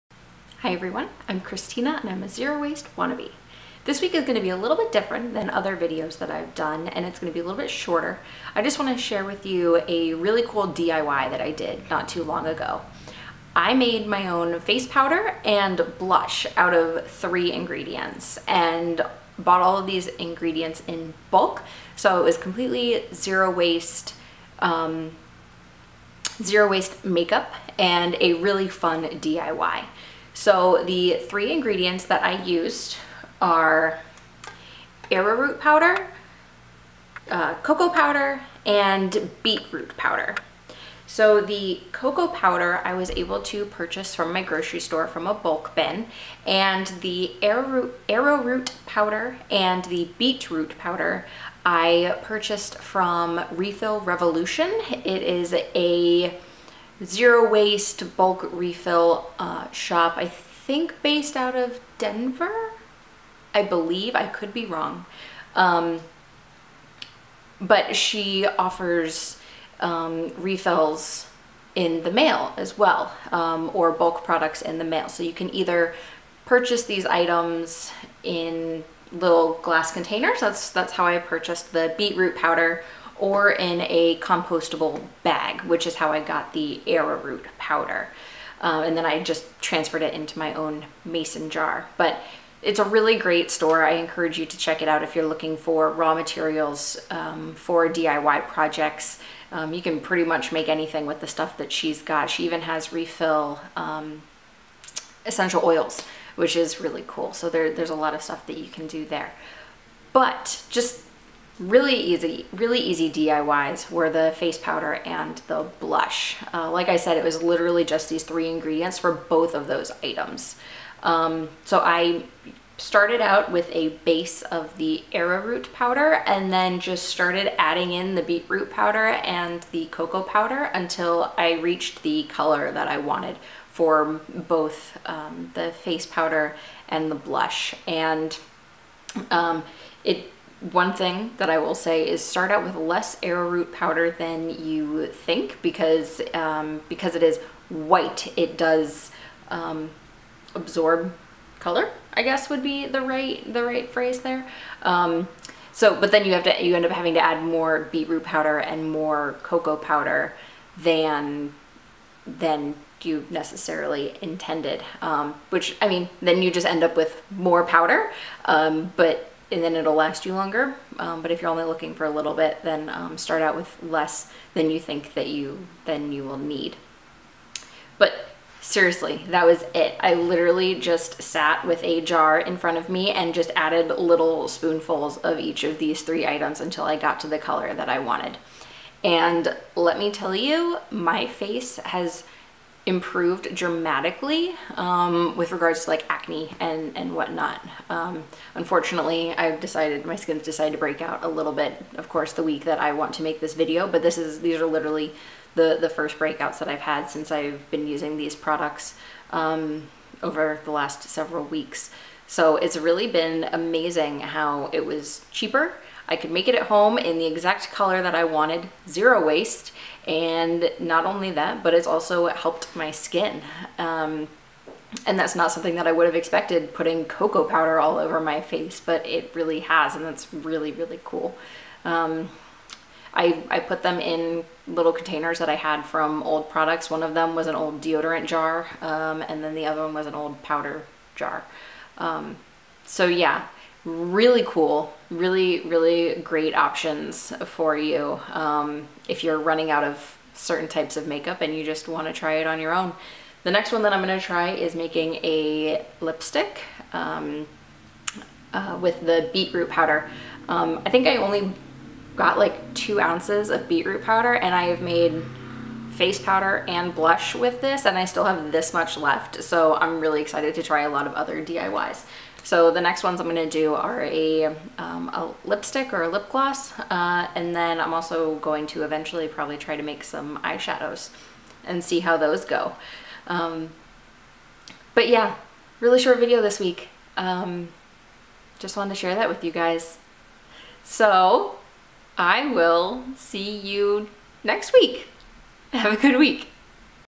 Zero Waste DIY: Blush + Face Powder
In this video I'm sharing how I made my own blush and face powder using only three ingredients!  This DIY is super easy and has been amazing for my skin.